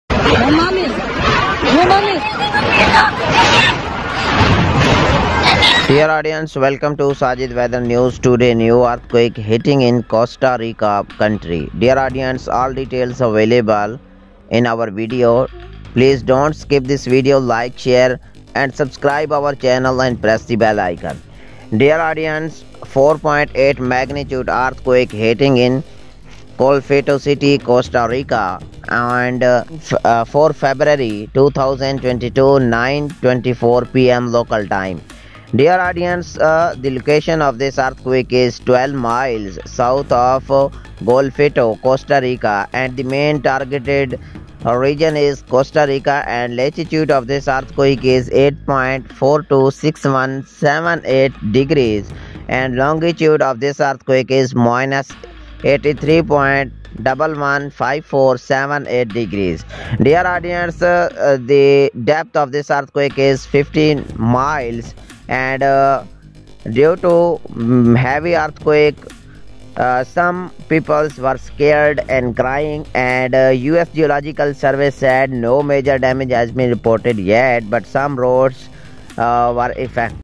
Costa Rica earthquake today | Heavy magnitude strikes Golfito, Costa Rica | weather today
Costa Rica Earthquake Today

Earthquake info:
4.8 magnitude hit Costa Rica on 4th February 2022 at 9:24 pm Costa Rica local time. depth 15 miles.

Cover :
Costa Rica earthquake 2022
Costa Rica live news
weather today
Sajid weather news